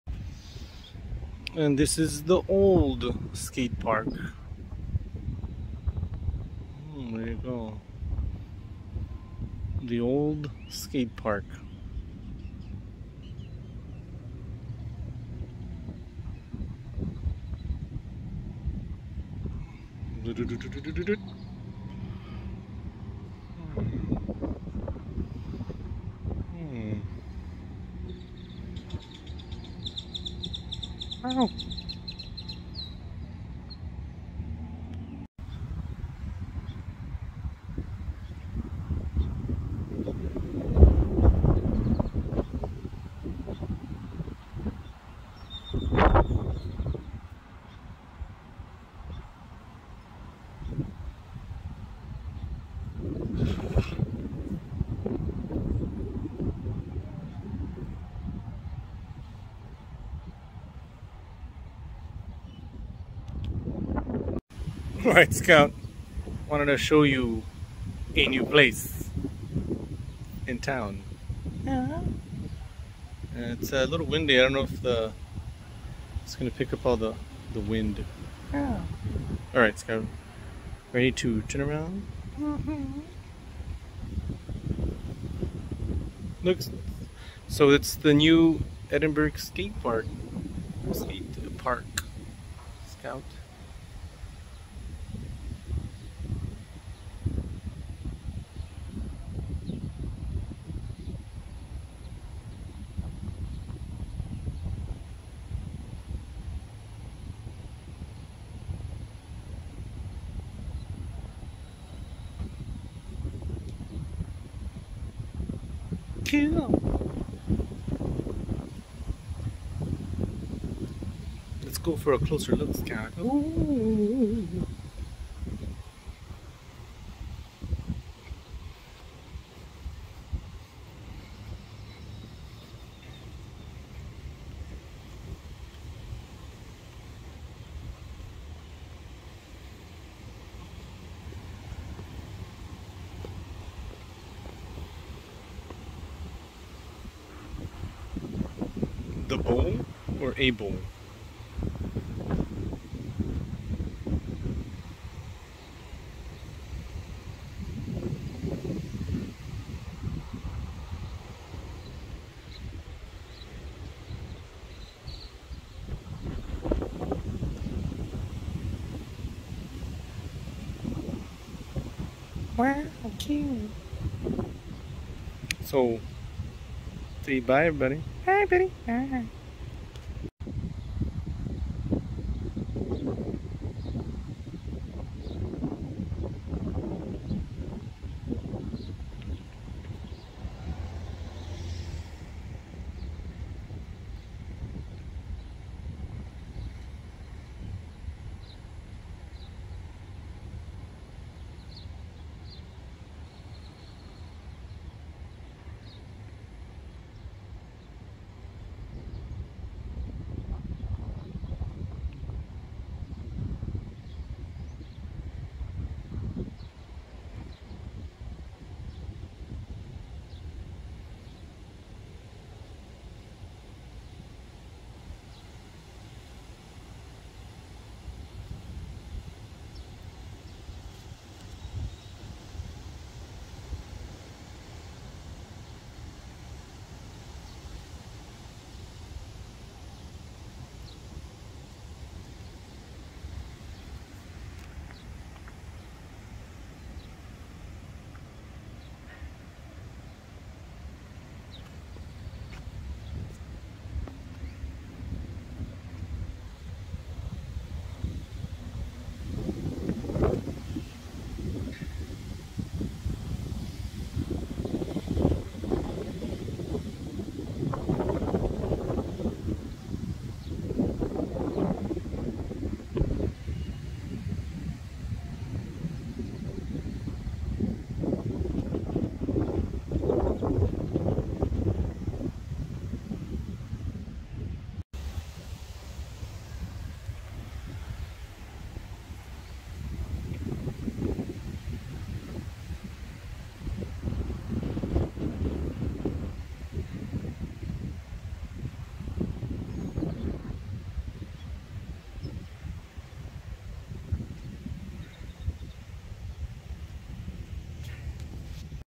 THE OLD and NEW Edinburg Skatepark with MY my pal Scout. Texas
THE OLD and NEW Edinburg Skatepark in this video with MY my pal Scout. Texas